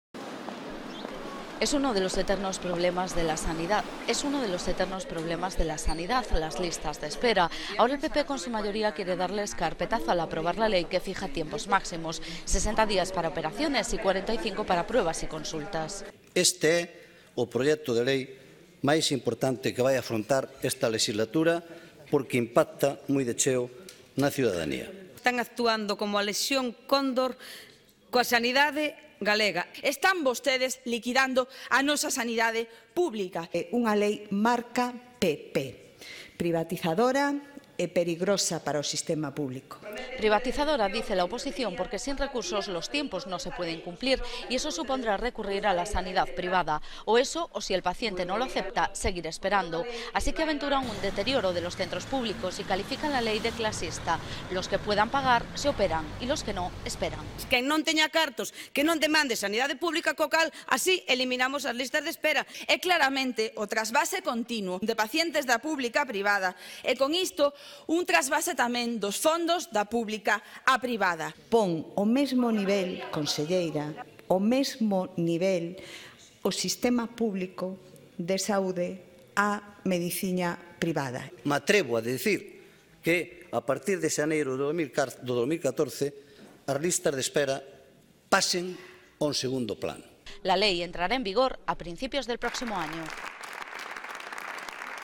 Aprobada la ley de Garantías sanitarias en un tenso pleno
Es uno de los eternos problemas de la sanidad: las listas de espera. Ahora el PP quiere darles carpetazo al aprobar la ley que fija tiempos máximos: 60 días para operaciones y 45 para pruebas y consultas.